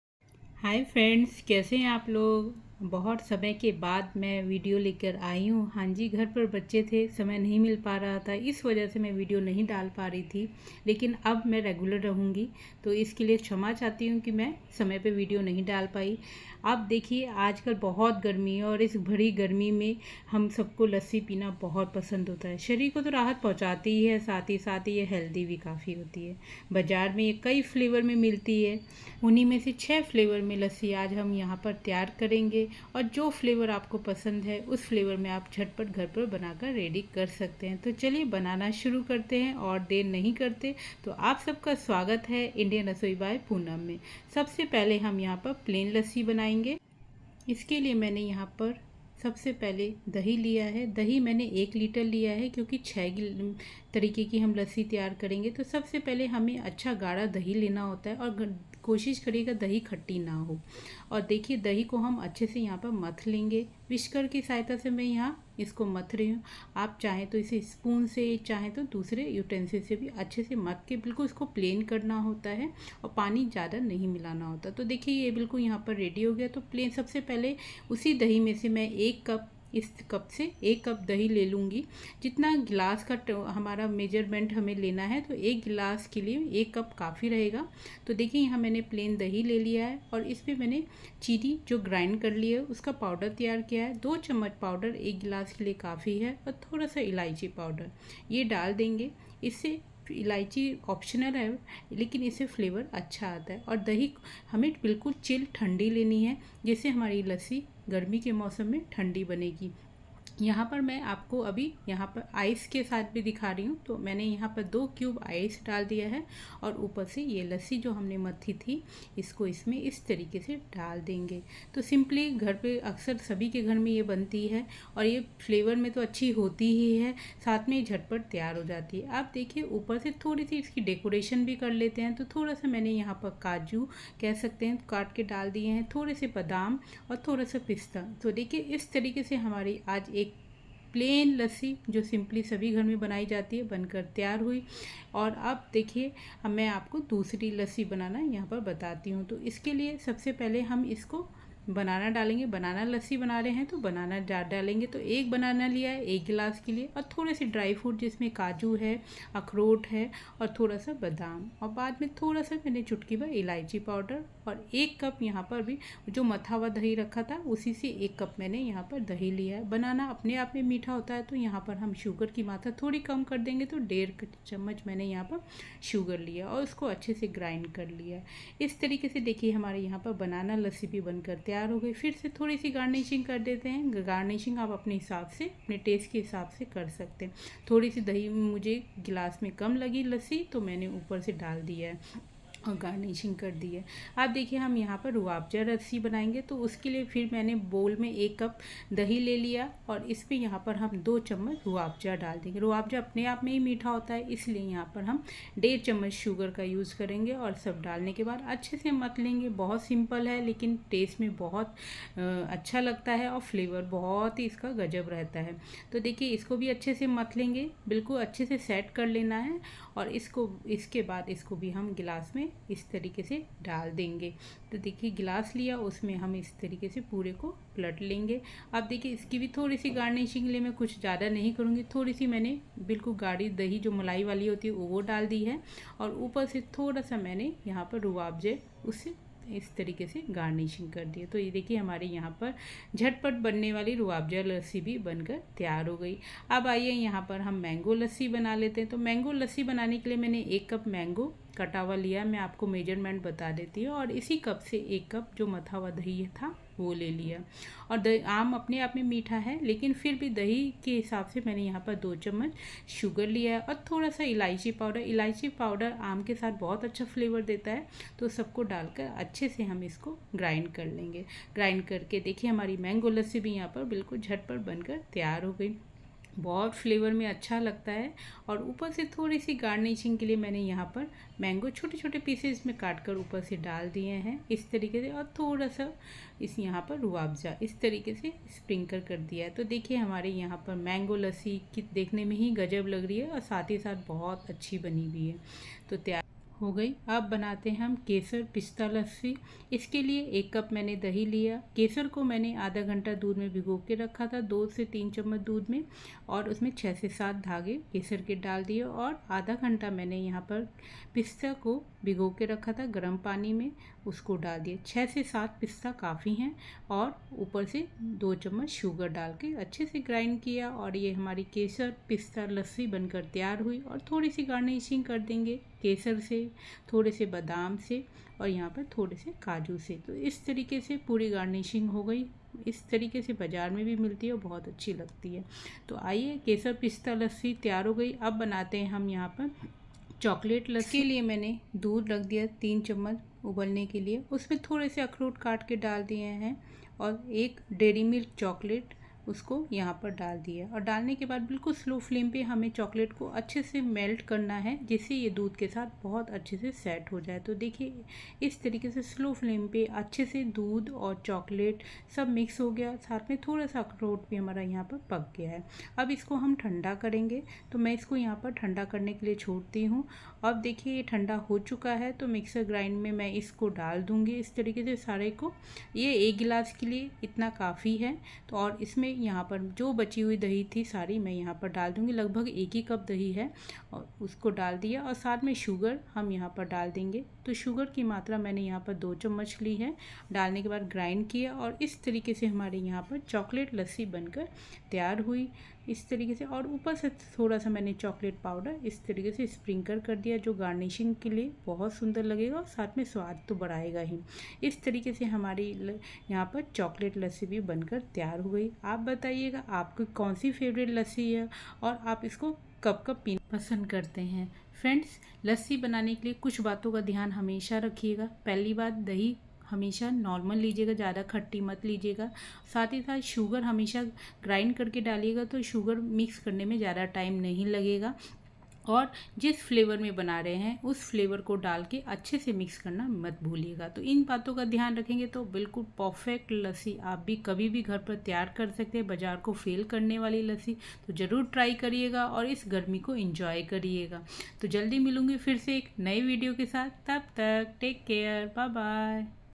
How to make Lassi I 6 अलग अलग स्वाद में त्यार होने वाली लस्सी l Summer Refreshing drink
Preparation time - 5 mins (for each Lassi)
Serving - 1

Mango lassi
Robja lassi
Chocolate lassi
Banana Lassi
Kesar Pista Lassi
Plain/Punjabi Lassi

Plain Lassi
Cardamon - 2 pinch

Chocolate lassi
Dairy Milk Chocolate -1(10rupee)
Walnut-2
Curd-1 Cup
Sugarpowder-2tbsp

Kesar Pista:
Pista -1 tbsp
Saffron (kesar)- 2 pinch

Rose lassi:
Rose syrup - 2 tbsp
Ice cubes
Water

Banana Lassi:
Curd - 1 cup
Banana -1
Walnut-2
Cashew-2
Walnut-2
Cashew-2
Sugar Powder-2tbsp
Cashew -4
Almond-4
Sugar - 1.5 tbsp
Ice cubes(Optional)

mango lassi:
Curd -1 cup
mango (ripe)- 1/2 cup
Ice cubes(Optional)
Water

Plain lassi-
ice cubes
water